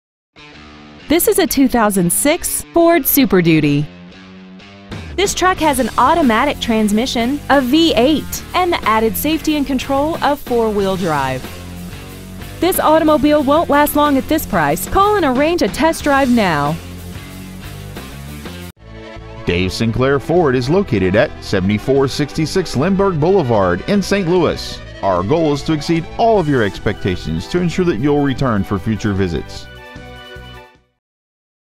2006 Ford Super Duty Oakville MO
Year : 2006
 Make : Ford
 Model : Super Duty
 Engine : 6L
 Trans . : automatic
 Exterior : Oxford White - (White)
 Miles : 74,658
 Interior : Medium Flint Cloth
 Stock : 4026102

 7466 Dave Sinclair Memorial Highway

 2006 Ford Super Duty St. Louis MO